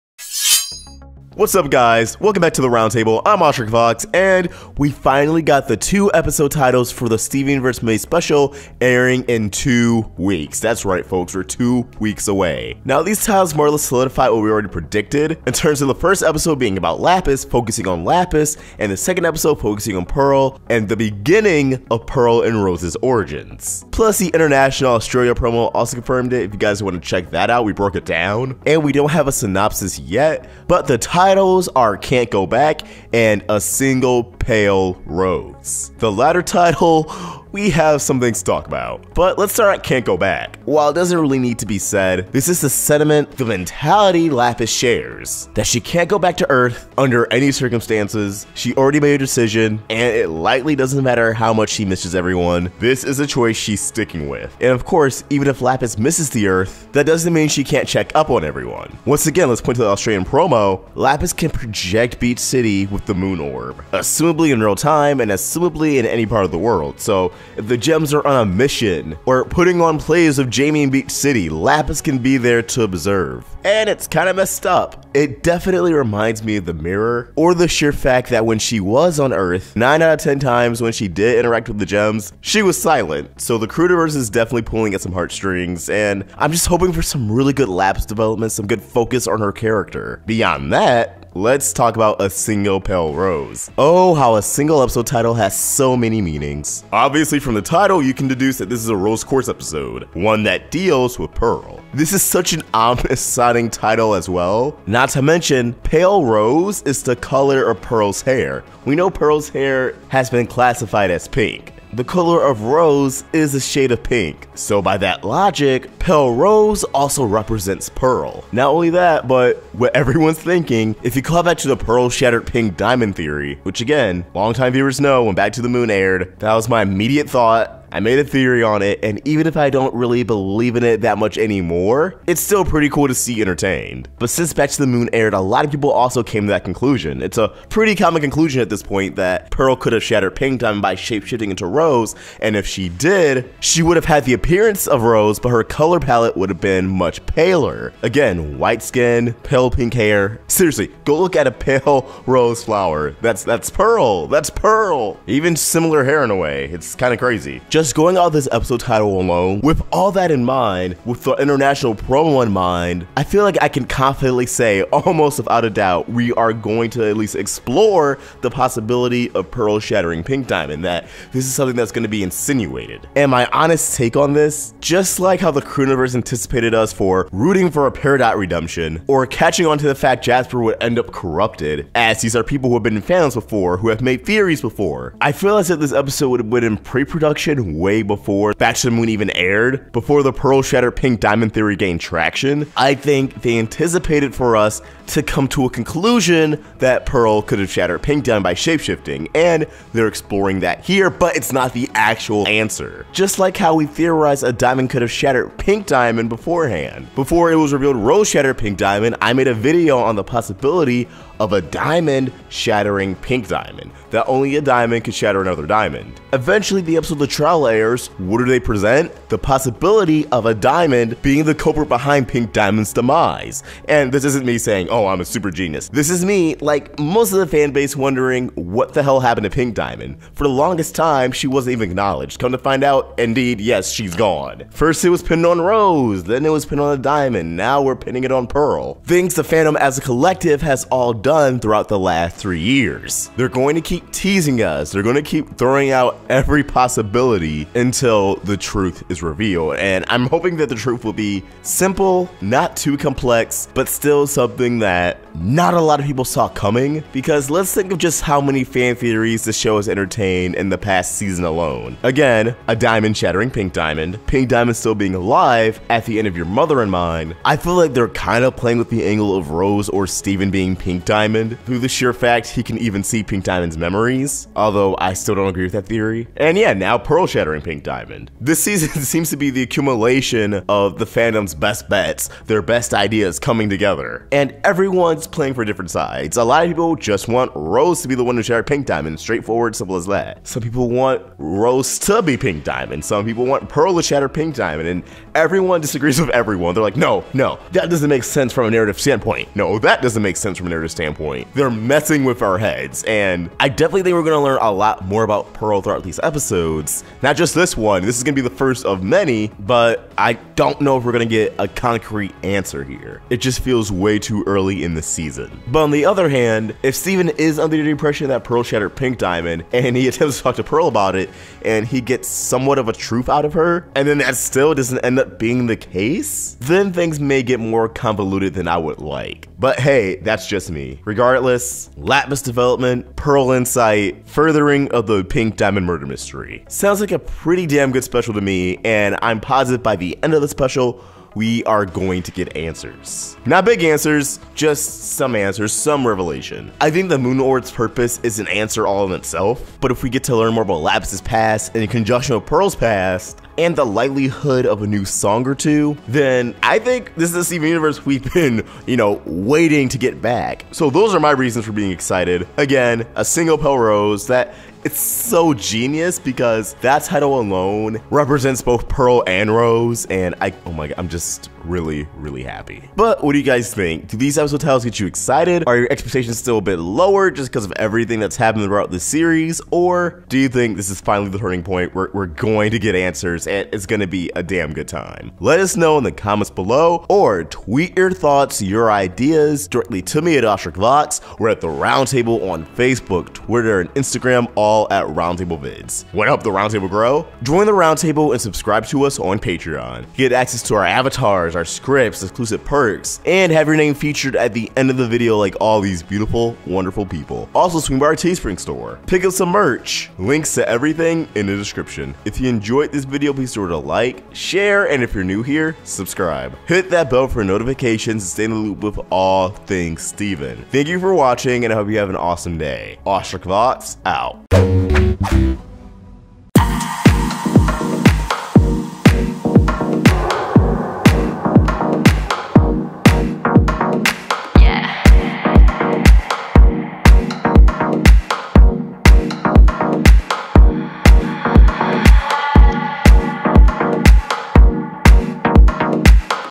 Did Pearl Shapeshift Into Rose Quartz? - Steven Universe May Special Titles REVEALED!
Brand new Steven Universe episode titles reveal the May special and allude to something much more ominous between Pearl and Rose Quartz! Let's discuss!

CREDITS

Written by:
Hosted by:
Edited by:
Produced by: